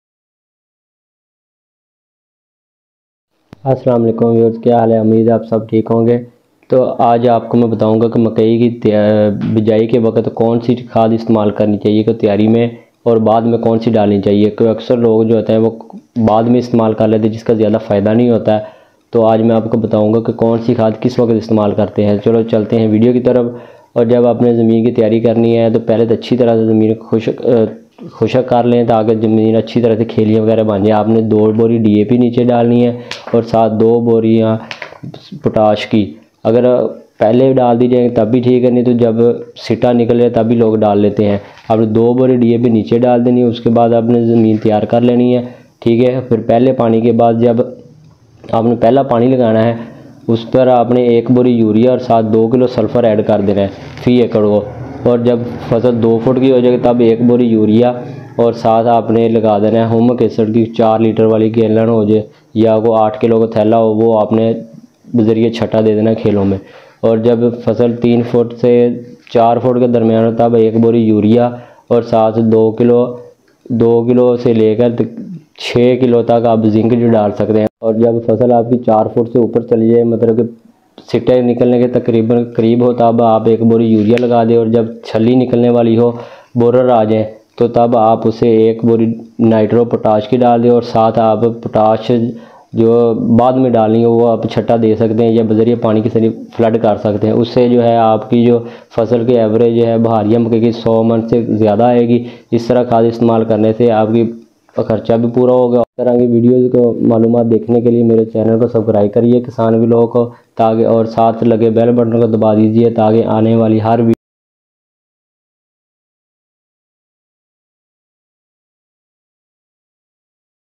sprin maiz fertilizer for guidlines, baharia make ki guidlines
Baharia make ki fertilization spring maiz ko kn kn c khad dalni chaey  Spring maiz ko DAP Urea nitro potash Fmc SOP Jaffer Brothers ki Solo Potash,|@kisanvlogofficial